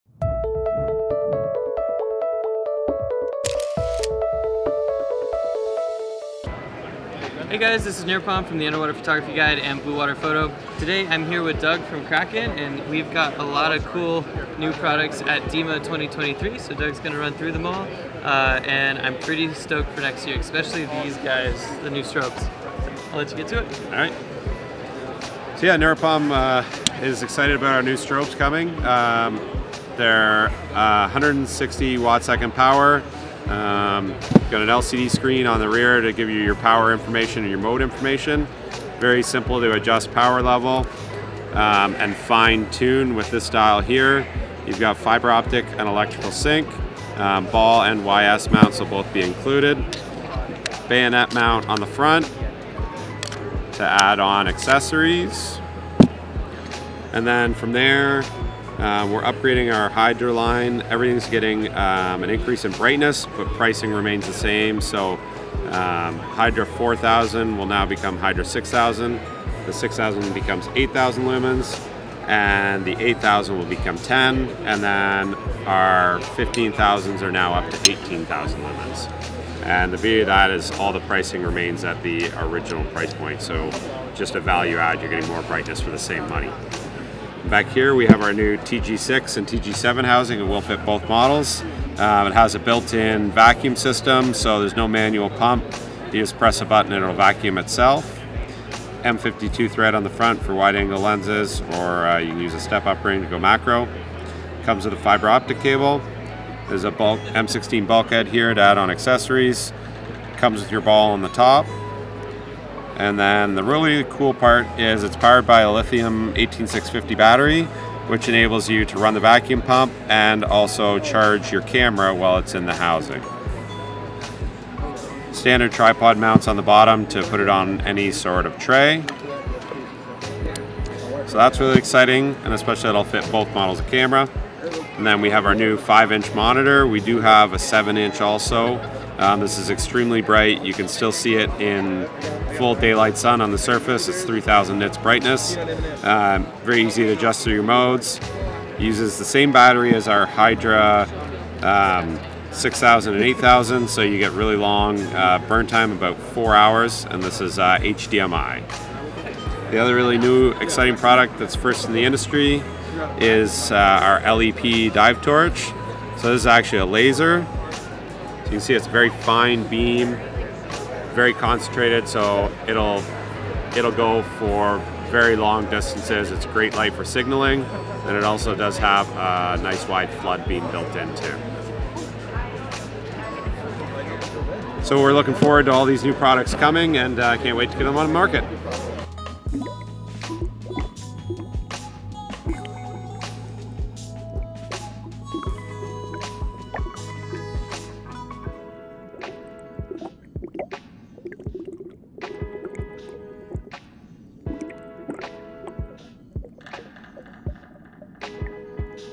Kraken KS-160 Strobes, TG-6 Housing, 5.5 inch ultra-bright monitor, new Hydra lights | DEMA 2023
We joined Doug at Kraken Sports during DEMA 2023 to see what's new at Kraken - including the Kraken KS-160 strobes, a new 5.5" inch ultra-bright monitor, and new hydra lights.